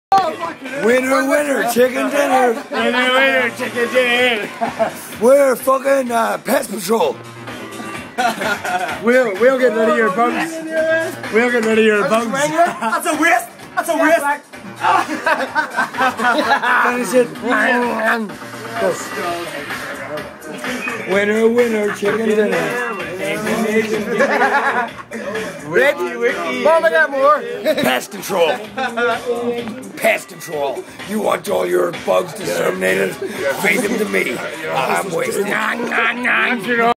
Pest control
Me eating strange Australian bugs :/